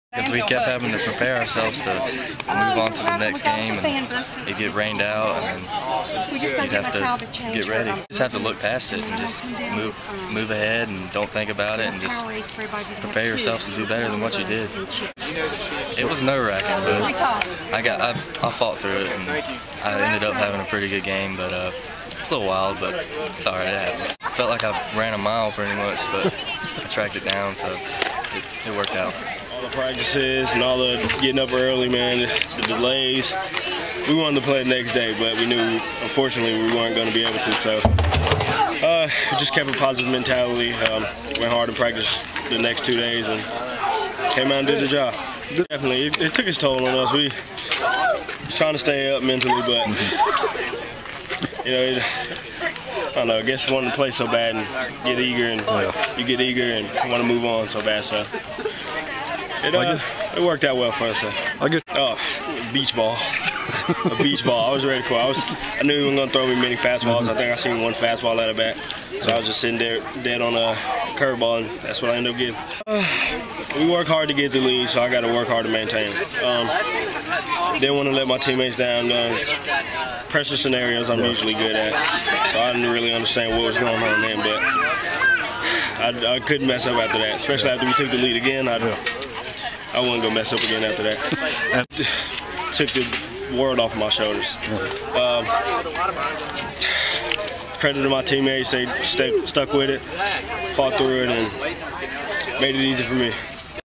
winder-barrow
Winder-Barrow's Drew Dunagan and Travis Demeritte talk about Tuesday's playoff win against South Paulding.